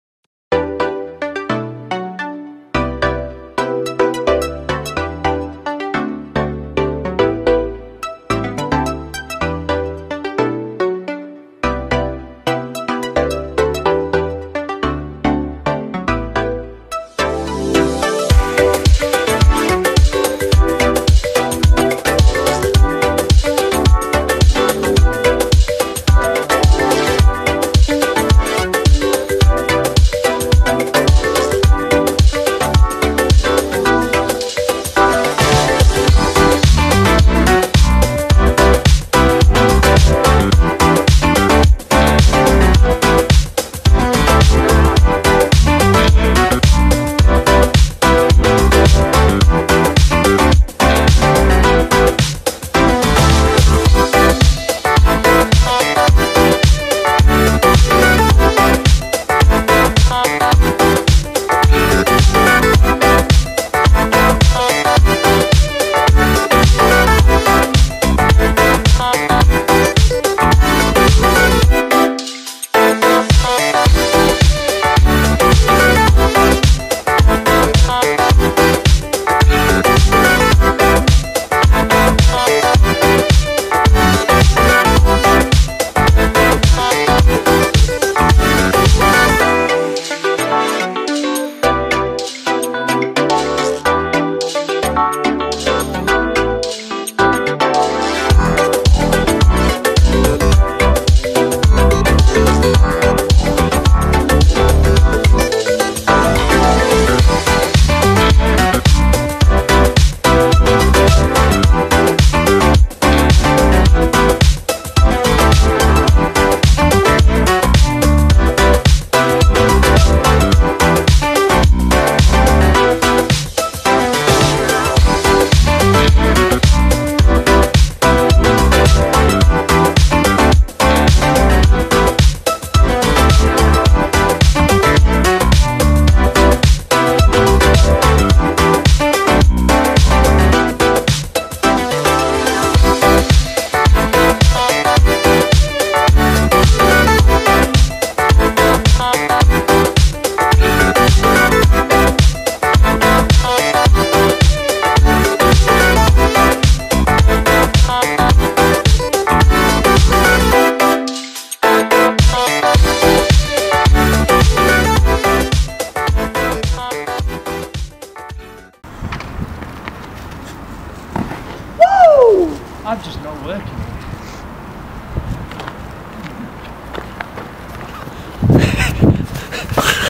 Spring Time 15
Spring is here and we're enjoying the weather in little old Mansfield. With 2 weeks of filming a small video of the recent jams.
Athletes: Dizzi Valentine, Luke Madigan, Andrew Stanley, Dom Piotrowski, Craig Canning and friends.
Filmed with: Canon 600D + GoPro Hero
Filmed and edited by: Shift - Dzuy Southam